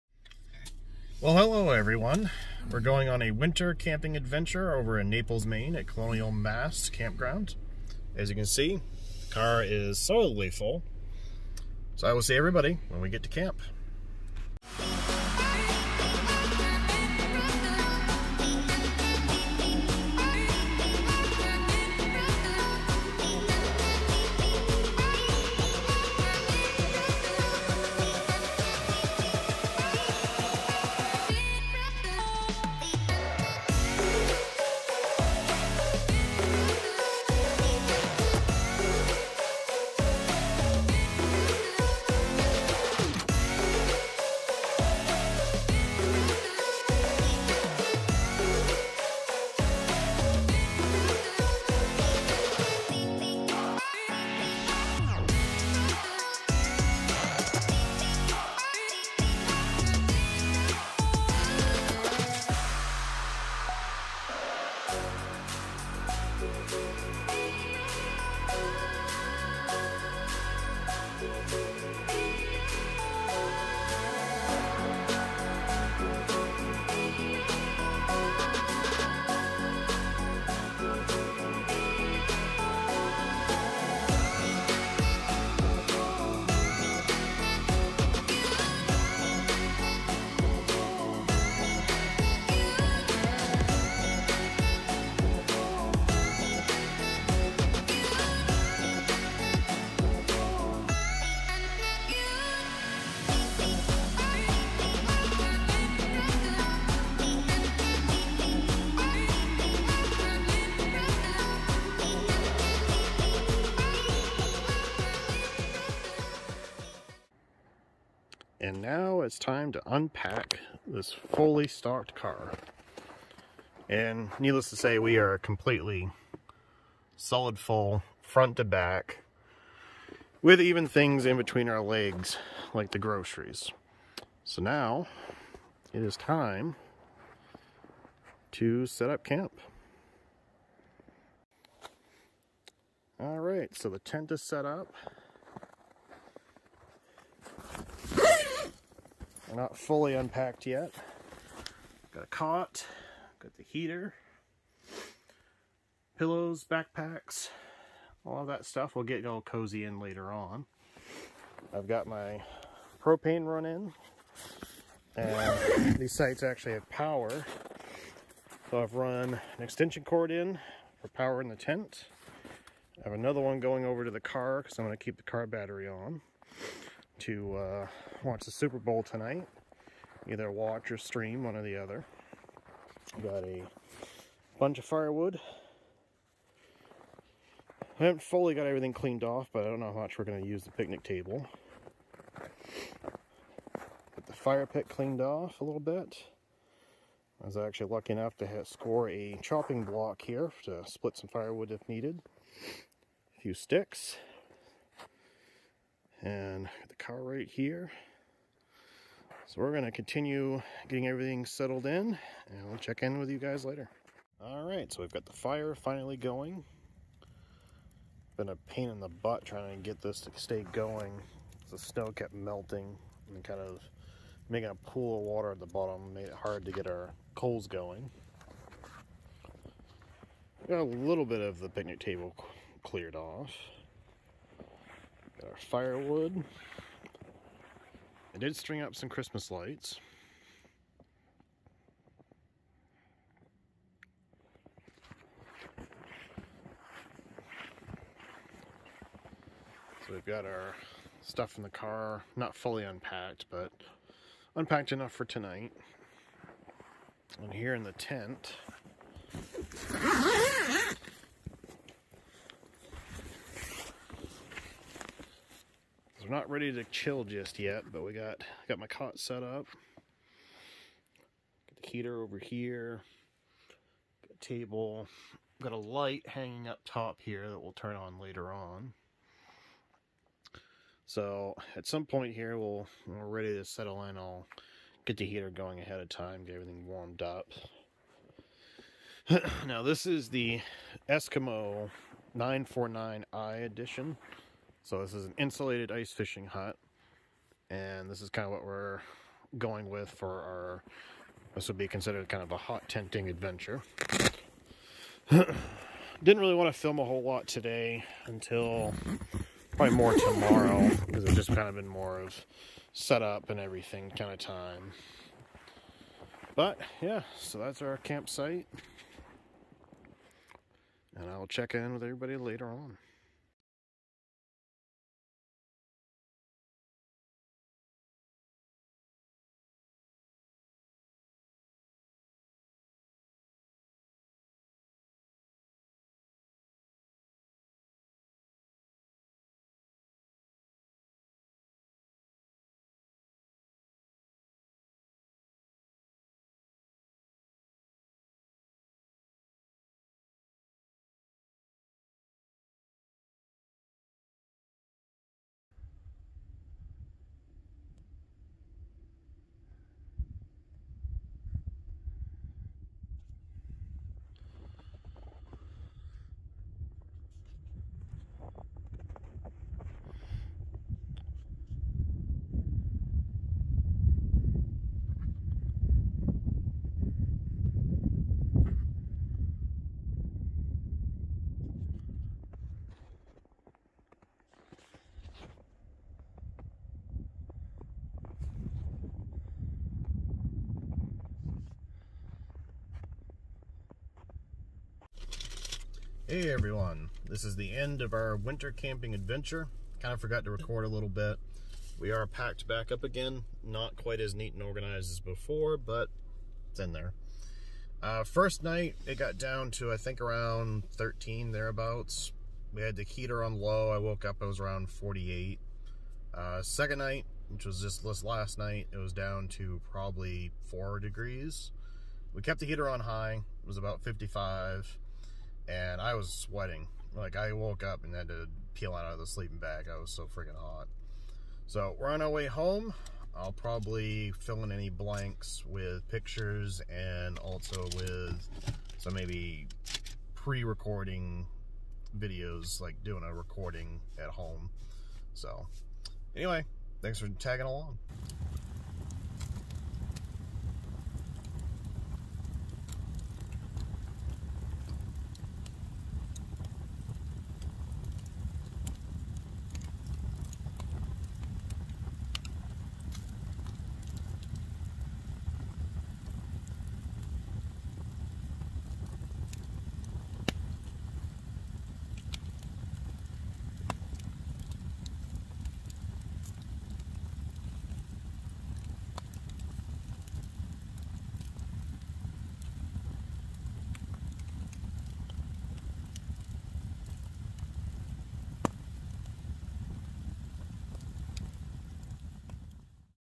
First time "Hot Tenting" in an Ice Fishing Shack!
First time my girlfriend and I have gone "Hot Tenting" in an Eskimo ice fishing shack! We stayed at the Colonial Mast Campground in Naples Maine, we also went ice fishing on Long Lake also in Naples Maine!

All video and pictures were taken on a Samsung Galaxy S21 Ultra, if anyone wants to know what I use for gear let me know and I can supply some links.